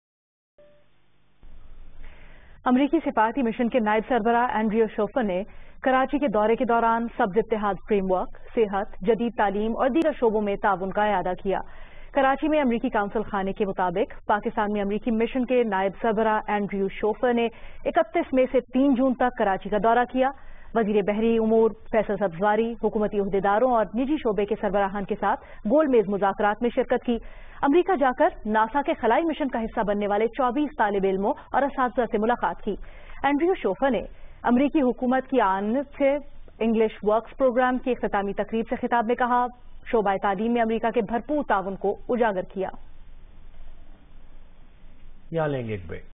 U.S. Deputy Chief of Mission Schofer Promotes “Green Alliance” Framework, Health, and Education
U.S. Deputy Chief of Mission Schofer Promotes “Green Alliance” Framework, Health, and Education in Karachi